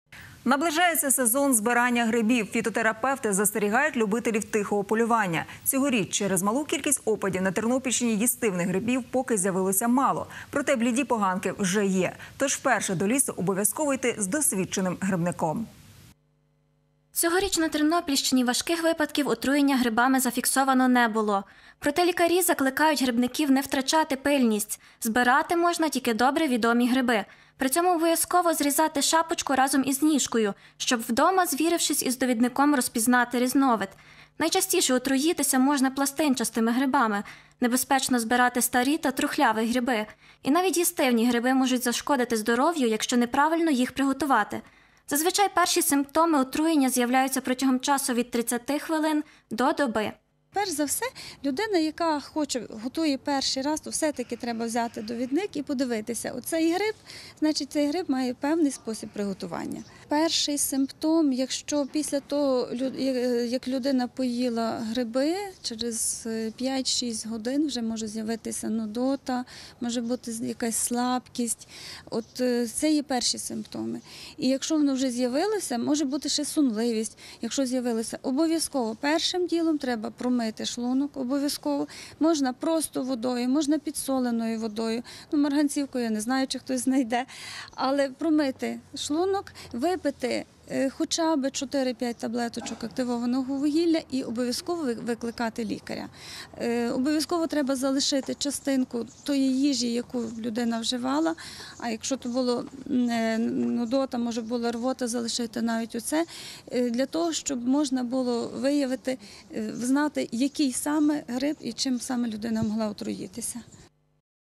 Поради грибникам-аматорам
Наближається сезон збирання грибів. Фітотерапевти застерігають любителів «тихого полювання» - цьогоріч через малу кількість опадів на Тернопільщині їстівних грибів поки з’явилося мало, проте бліді поганки вже є. Тож, вперше до лісу обов’язково йти з досвідченим грибником.